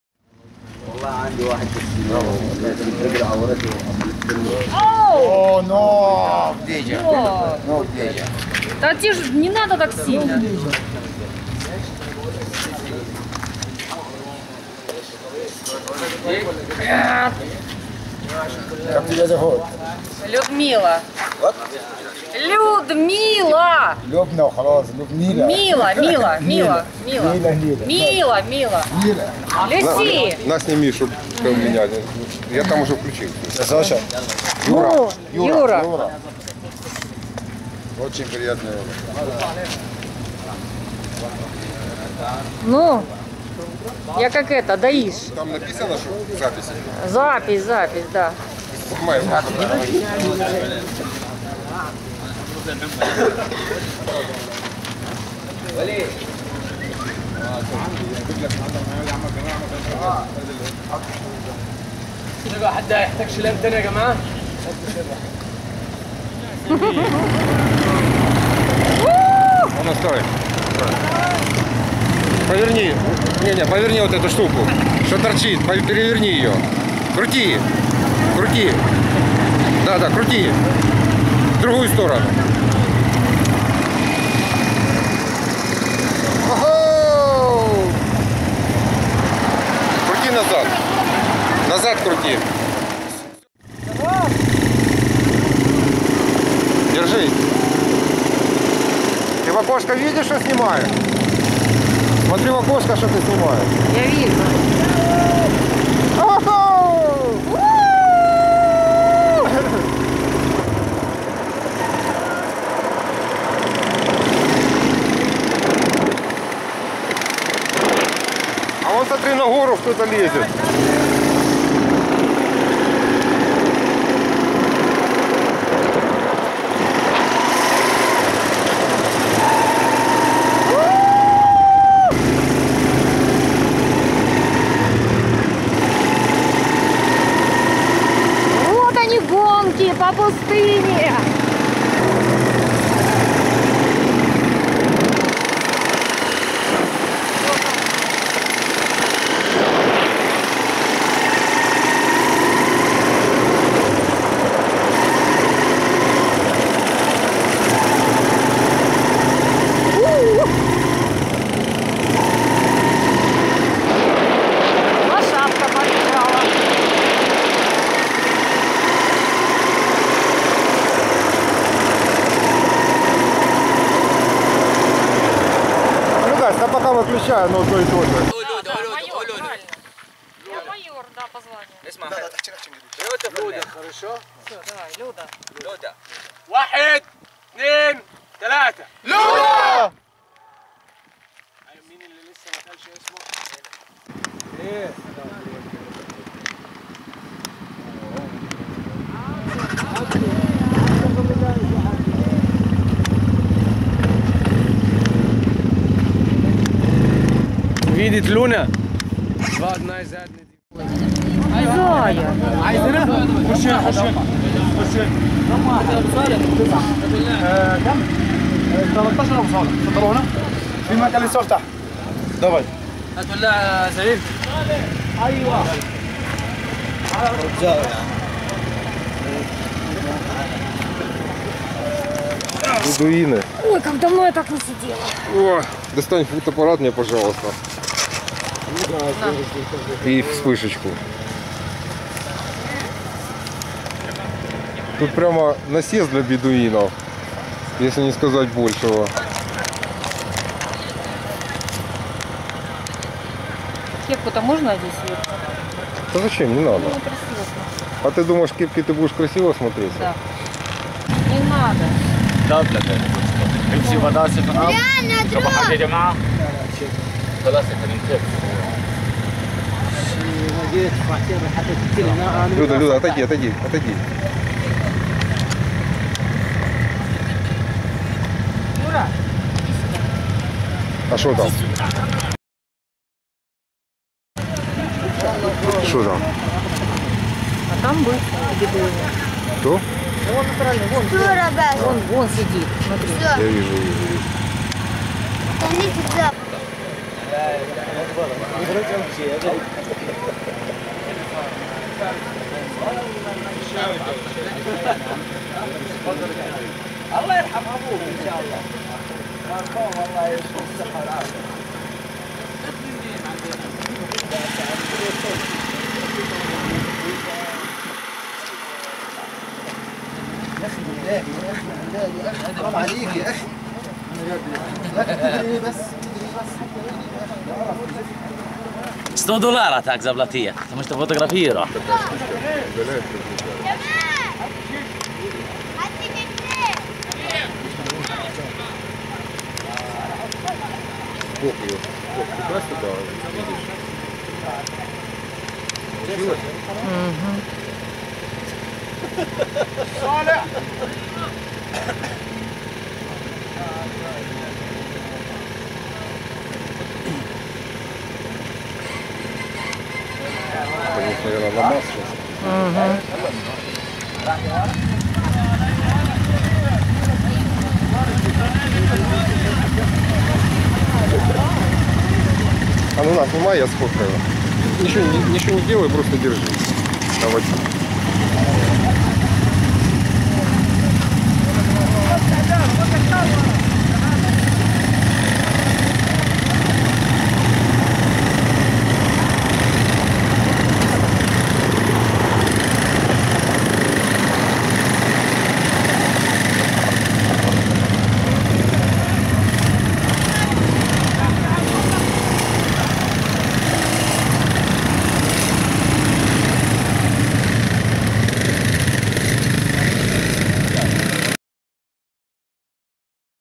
Египет,Шарм-ель-шейх,мото-сафари, Egypt, Sharm el-Sheikh, moto-safari
видео показывает участие в мото-сафари по пустыне в Египте Шарм-ель-шейхе, video shows participation in the Moto desert safari in Egypt, Sharm-el-Sheikh,